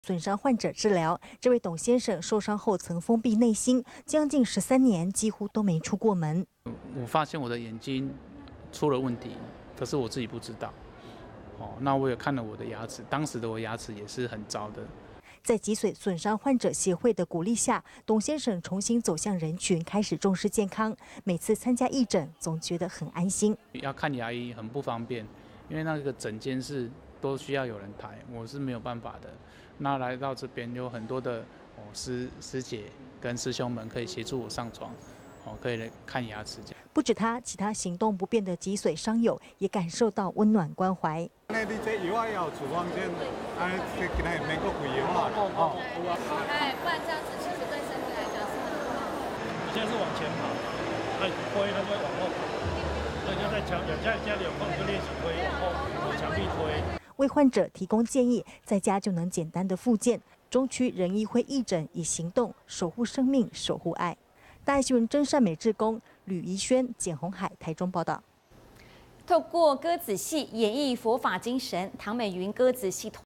叮囑脊傷患吃藥復健 醫者父母心
將近十三年，幾乎沒出門，這是一名脊髓損傷病友，真實情況。因為要他們走向人群並不容易。慈濟人醫會，為脊髓損傷病友，舉辦義診。要呵護的是身體，也是心靈。

慈濟人醫會醫師：「你有受過什麼傷嗎。」

人醫細心問診，為脊髓損傷患者治療，這位董先生，受傷後曾封閉內心，將近13年，幾乎都沒出門。

脊髓損傷病友 董先生：「我發現我的眼睛出了問題，可是我自己不知道  ，我也看了我的牙齒，當時我的牙齒也是很糟的。」

在脊髓損傷者協會的鼓勵下，董先生重新走向人群，開始重視健康，每次參加義診，總覺得很安心。

脊髓損傷病友 董先生：「要看牙醫很不方便，因為診間都需要有人抬，我是沒有辦法的 ，來到這邊 有很多的師姊師兄，可以協助我上(診療)床 看牙齒。」

不只他，其他行動不便的脊髓傷友，也感受到溫暖關懷。

慈濟人醫會醫師：「你這藥還有處方箋 ，今天不用再開藥了，對 不然(吃太多藥)對身體不好。」

慈濟人醫會醫師：「你現在是往前跑，你推它 就會往後跑，在家裡有空你就練習推 往牆壁推。」

為患者提供建議，在家能簡單復健，中區人醫義診，以行動守護生命守護愛。

真善美志工 呂怡萱 簡鴻海 台中報導